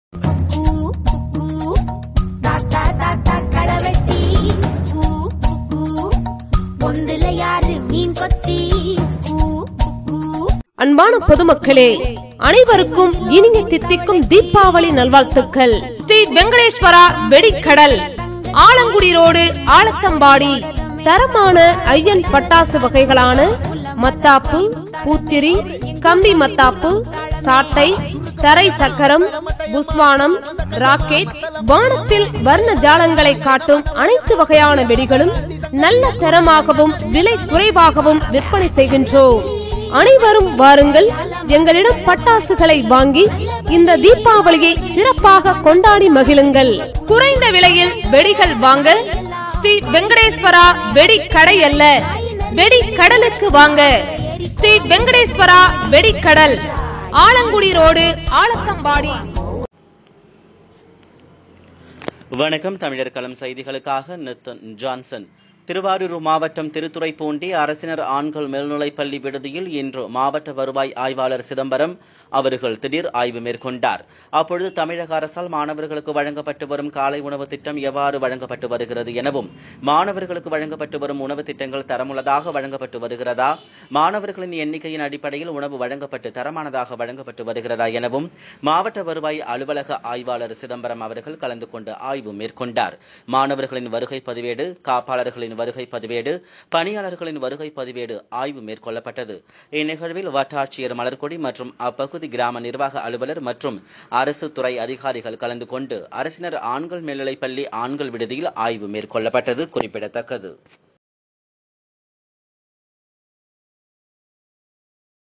ஆண்கள் மேல்நிலைப்பள்ளி விடுதியில் மாவட்ட வருவாய் ஆய்வாளர் சிதம்பரம் அவர்கள்திடீர்ஆய்வு மேற்கொண்டார்
திருவாரூர் மாவட்டம் திருத்துறைப்பூண்டி அரசினர் ஆண்கள் மேல்நிலைப்பள்ளி விடுதியில்  மாவட்ட வருவாய் ஆய்வாளர் சிதம்பரம் அவர்கள் திடீர் ஆய்வு மேற்கொண்டார் .
அப்பொழுது தமிழக அரசால் மாணவர்களுக்கு வழங்கப்பட்டு வரும் காலை உணவு திட்டம் எவ்வாறு வழங்கப்பட்டு வருகிறது எனவும்.
 மாணவர்களுக்கு வழங்கப்பட்டு வரும் உணவு திட்டங்கள் தரமுள்ளதாக வழங்கப்பட்டு வருகிறதா மாணவர்களின் எண்ணிக்கையின் அடிப்படையில் உணவு வழங்கப்பட்டு தரமானதாக வழங்கப்பட்டு வருகிறதா எனவும் மாவட்ட வருவாய் அலுவலக ஆய்வாளர் சிதம்பரம் அவர்கள் கலந்துகொண்டு ஆய்வு மேற்கொண்டார் .
மாணவர்களின் வருகை பதிவேடு காப்பாளர்களின் வருகை பதிவேடு பணியாளர்களின் வருகை பதிவேடு ஆய்வு மேற்கொள்ளப்பட்டது இந்நிகழ்வில் வட்டாட்சியர் மலர்கொடி மற்றும் அப்பகுதி கிராம நிர்வாக அலுவலர் மற்றும் அரசு துறை அதிகாரிகள் கலந்து கொண்டு அரசினர் ஆண்கள் மேல்நிலைப்பள்ளி  ஆண்கள் விடுதியில் ஆய்வு  மேற்கொள்ளப்பட்டது.